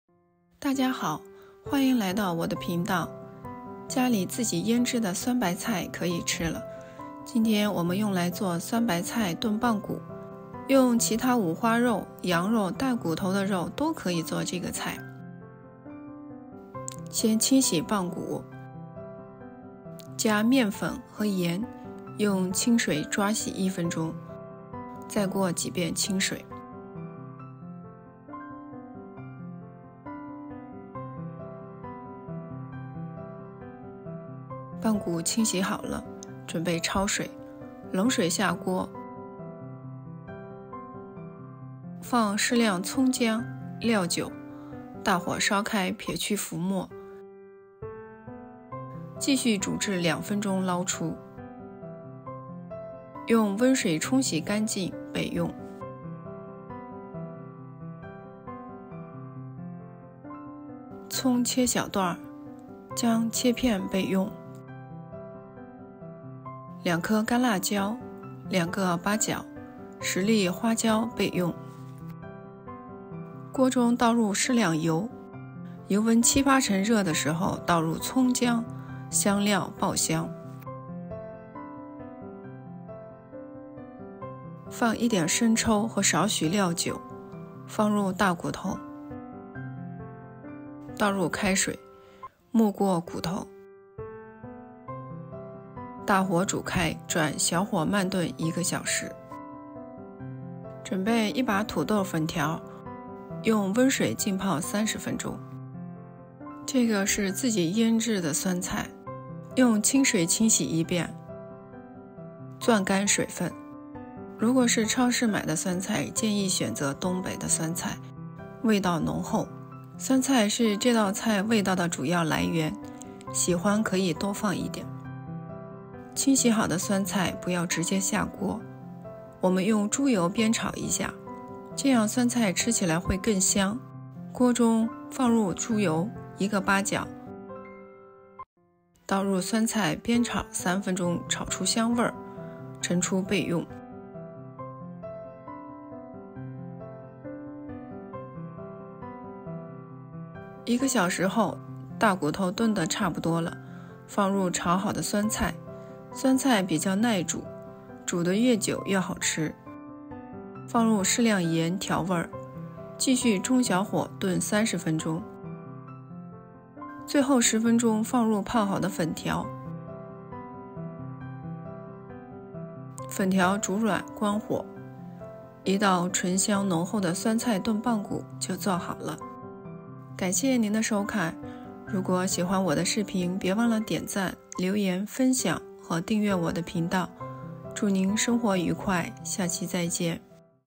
用自制酸菜配棒骨做一道热气腾腾的传统美味，鲜香酸爽❗️
酸菜炖棒骨是一道东北名菜，酸白菜的吃法很多，可以做酸菜白肉、酸菜饺子、酸菜炖羊肉。羊肉和酸菜天生一对，搭配在一起非常好吃。做羊肉酸菜油要多，不然香气出不来。先炸几个辣椒，接着将羊肉和酸菜大火煸炒一下，最后放入泡好的粉丝炒匀出锅。味道酸香，带一些羊膻气，非常适合用来拌饭。
食材：
猪棒骨     1500克
酸白菜     500克
土豆粉条 1把
葱姜         适量
干辣椒     2个
八角        2个
花椒       10粒
生抽       10克
料酒       5克
盐          2克